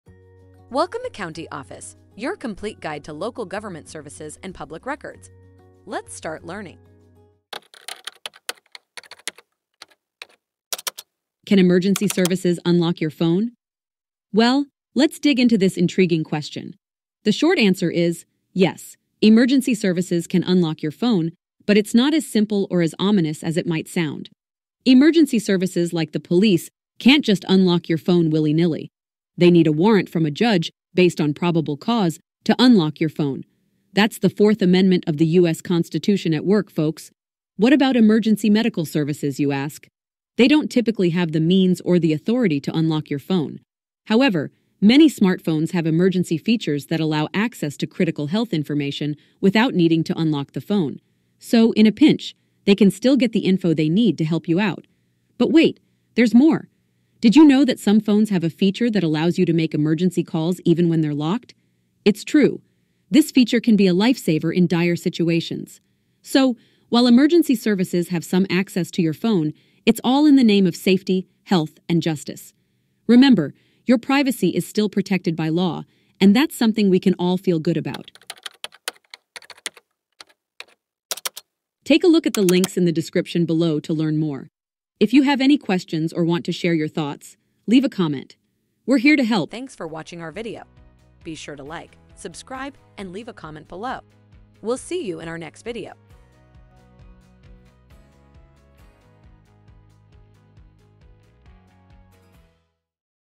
Can Emergency Services Unlock Your Phone? - CountyOffice.org
Can Emergency Services Unlock Your Phone? Ever thought about whether emergency services can unlock your phone and how they do it? In this eye-opening video, we delve into this fascinating question and provide you with all the crucial information you need to know.

We'll begin by discussing the circumstances under which emergency services, like the police, can unlock your phone. We'll explain the legal requirements they must meet and how the Fourth Amendment of the U.S. Constitution protects your privacy.

Next, we'll explore the role of emergency medical services and whether they have the authority to unlock your phone. We'll also discuss the emergency features available on many smartphones that provide access to critical health information without unlocking the device.

Finally, we'll reveal the life-saving feature that allows you to make emergency calls even when your phone is locked!